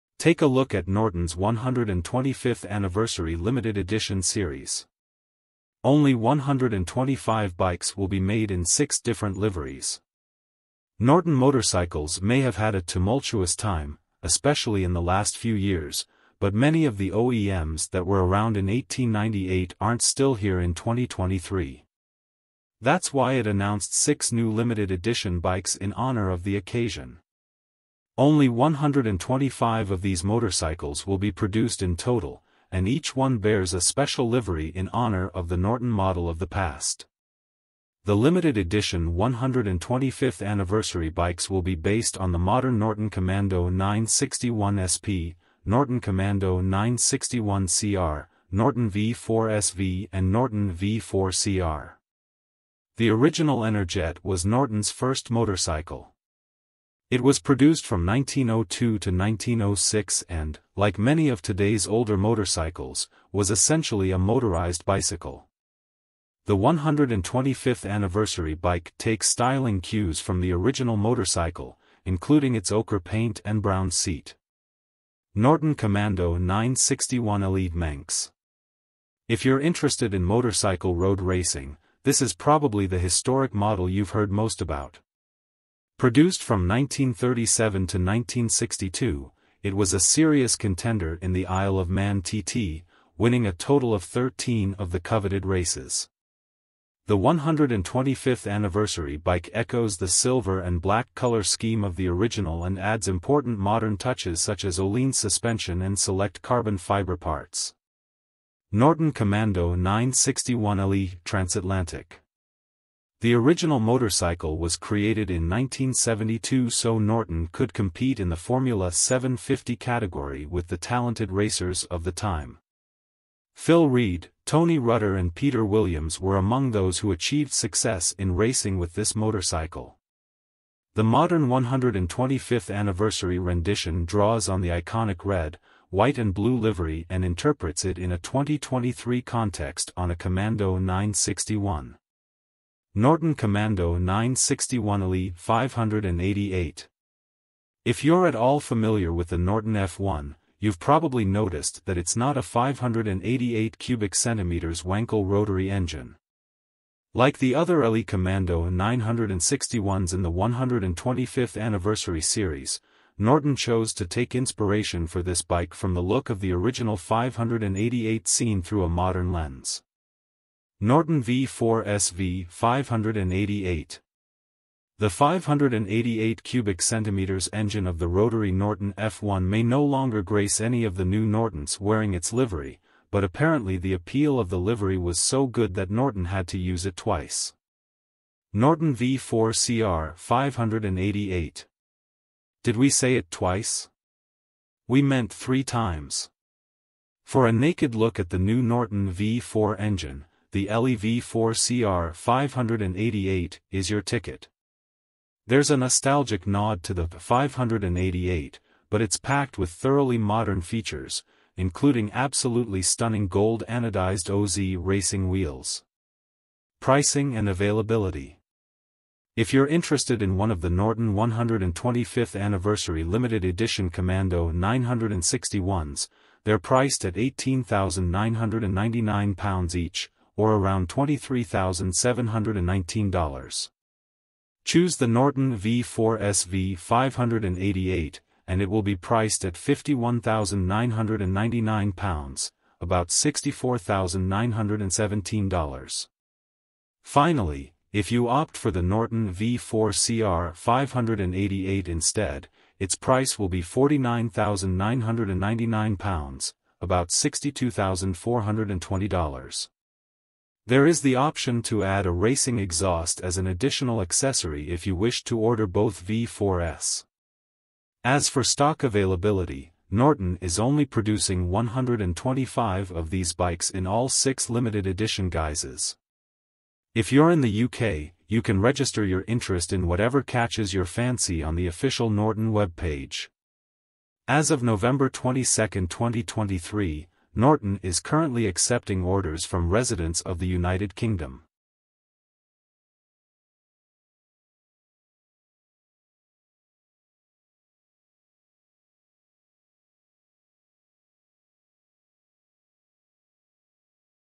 Take a Look at Norton's 125th Anniversary Series,New Norton 125th Anniversary Limited Edition Series
Only 125 bikes will be made in six different liveries.

Norton Motorcycles may have had a tumultuous time (especially in the last few years), but many of the OEMs that were around in 1898 aren't still here in 2023.

That's why it announced six new limited edition bikes in honor of the occasion. Only 125 of these motorcycles will be produced in total, and each one bears a special livery in honor of the Norton model of the past.

The limited edition 125th anniversary bikes will be based on the modern Norton Commando 961 SP, Norton Commando 961 CR, Norton V4SV and Norton V4CR.

The original Energette was Norton's first motorcycle. It was produced from 1902 to 1906 and, like many of today's older motorcycles, was essentially a motorized bicycle. The 125th Anniversary bike takes styling cues from the original motorcycle, including its ocher paint and brown seat.

Norton Commando 961 LE 'Manx'
If you're interested in motorcycle road racing, this is probably the historic model you've heard most about. Produced from 1937 to 1962, it was a serious contender in the Isle of Man TT, winning a total of 13 of the coveted races. The 125th Anniversary bike echoes the silver and black color scheme of the original and adds important modern touches such as Öhlins suspension and select carbon fiber parts.

Norton Commando 961 LE 'Transatlantic'
The original motorcycle was created in 1972 so Norton could compete in the Formula 750 category with the talented racers of the time. Phil Read, Tony Rutter and Peter Williams were among those who achieved success in racing with this motorcycle. The modern 125th Anniversary rendition draws on the iconic red, white and blue livery and interprets it in a 2023 context on a Commando 961.

Norton Commando 961 LE '588'
If you're at all familiar with the Norton F1, you've probably noticed that it's not a 588cc Wankel rotary engine. Like the other LE Commando 961s in the 125th anniversary series, Norton chose to take inspiration for this bike from the look of the original 588 seen through a modern lens.

Norton V4SV '588'
The 588cc engine of the rotary Norton F1 may no longer grace any of the new Nortons wearing its livery, but apparently the appeal of the livery was so good that Norton had to use it twice.

Norton V4CR '588'
Did we say it twice? We meant three times. For a naked look at the new Norton V4 engine, the LE V4CR '588' is your ticket. There's a nostalgic nod to the '588, but it's packed with thoroughly modern features, including absolutely stunning gold anodized OZ Racing wheels.

If you're interested in one of the Norton 125th Anniversary Limited Edition Commando 961s, they're priced at £18,999 each (or around $23,719).

Choose the Norton V4SV '588' and it will be priced at £51,999 (about $64,917). Finally, if you opt for the Norton V4CR '588 instead, its price will be £49,999 (about $62,420).

There is the option to add a racing exhaust as an additional accessory if you wish to order both V4s.

As for stock availability, Norton is only producing 125 of these bikes in all six limited edition guises. If you're in the UK, you can register your interest in whatever catches your fancy on the official Norton web page. As of November 22, 2023, Norton is currently accepting orders from residents of the United Kingdom.